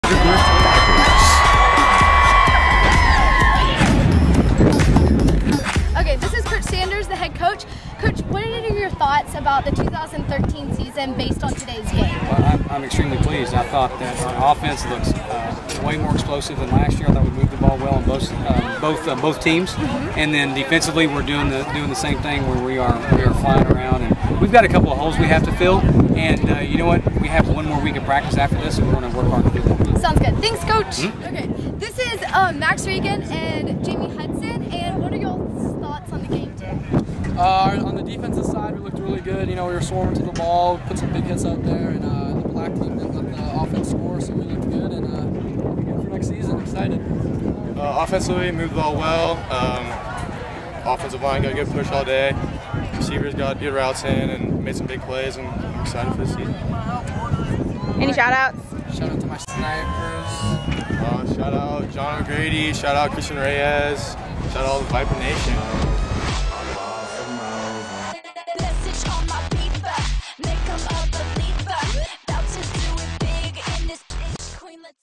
Vandegrift Vipers Football | Spring Camp | 2013
highschoolsports txhsfb 🏈 Viperpalooza drew one of its biggest crowds ever earlier today, as the Vandegrift Football team put on a show. i-Reporter Taylor Searels ('15) made her debut, talking with head coach Drew Sanders and other key Vipers. VV will continue spring camp through the end of next week.